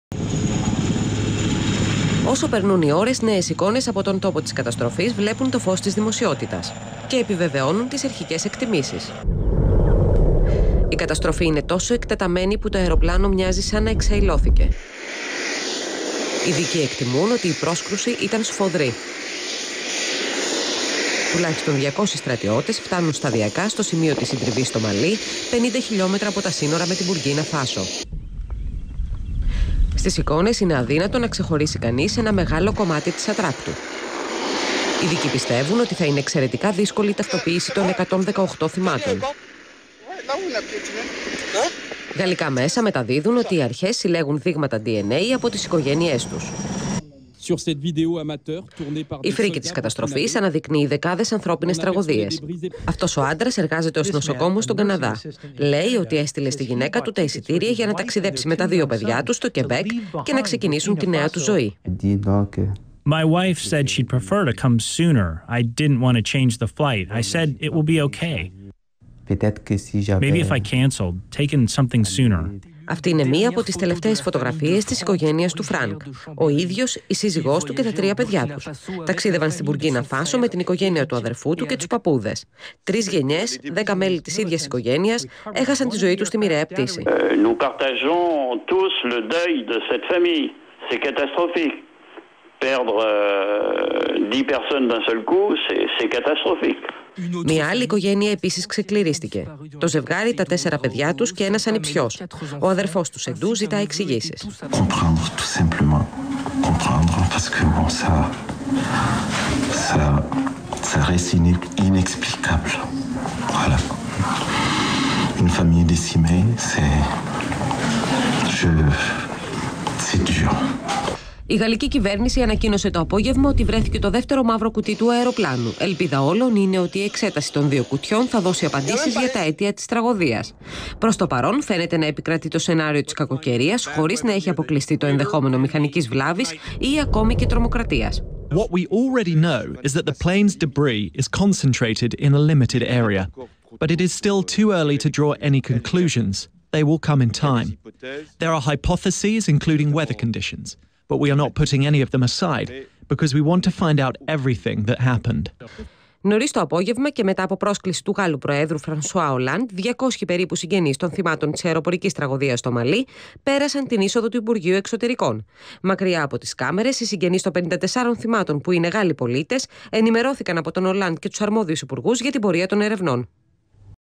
Νέα στοιχεία για την πτώση του αεροσκάφους στο Μαλί - MEGA ΓΕΓΟΝΟΤΑ
Σύμφωνα με νεότερα στοιχεία το αεροπλάνο των Αλγερινών Αερογραμμών σύρθηκε στο έδαφος και πήρε φωτιά. Γάλλοι ειδικοί στο σημείο της συντριβής.